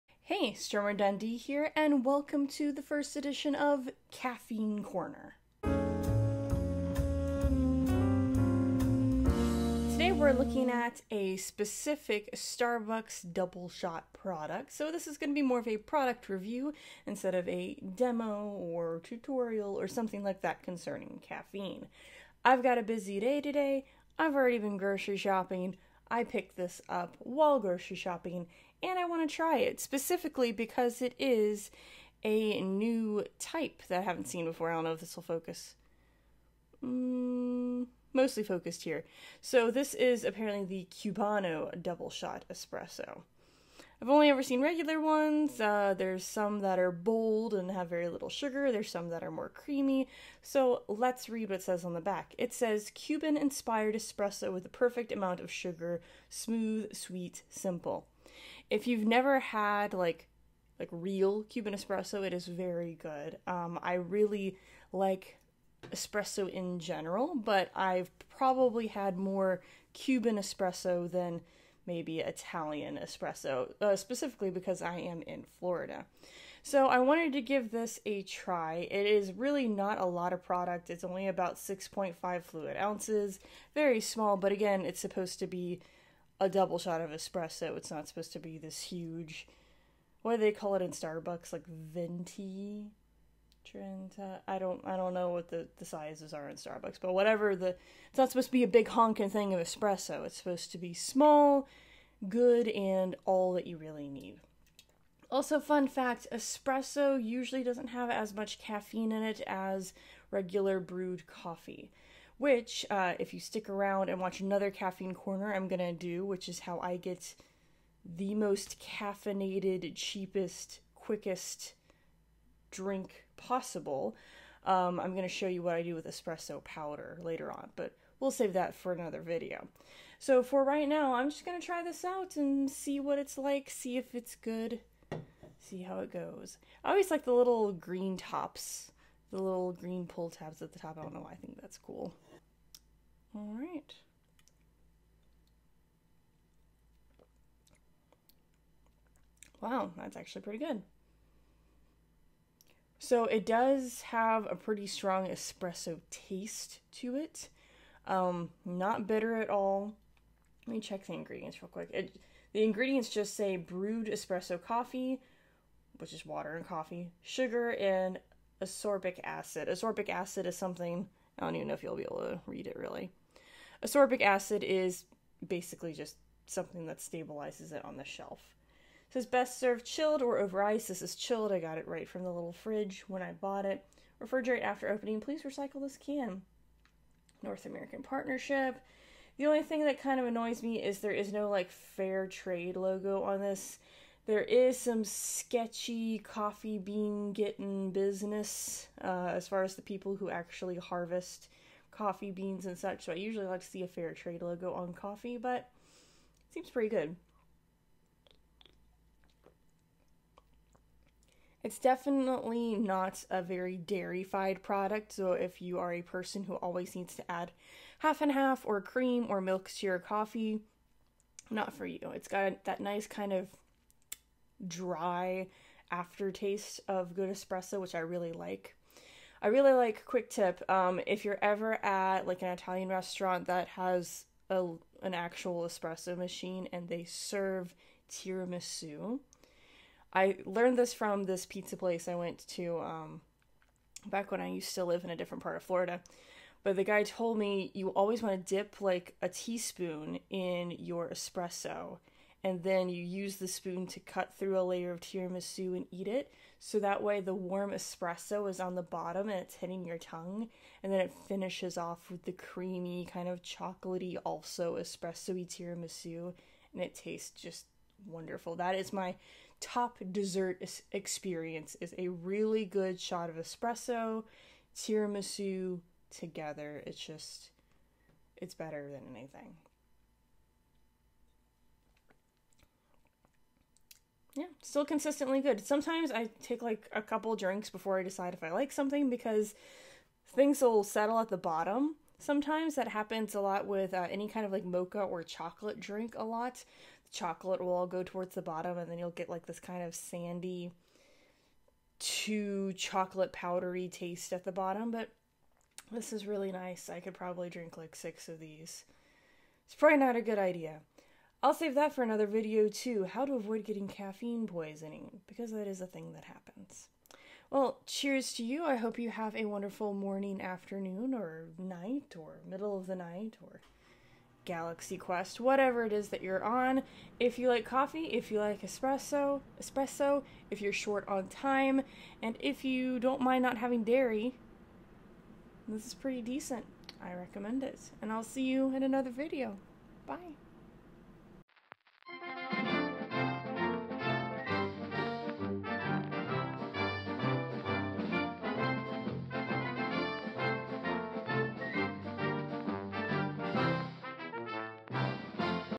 Caffeine Corner - CUBANO DOUBLESHOT Review
It's morning. I'm tired. I have a passion for canned coffee drinks. I only wish I could wake up to a fresh Cuban espresso every morning but sadly I cannot...so let's try the Starbucks Cubano Doubleshot Espresso and see if it's got the caffeinated goodness I need.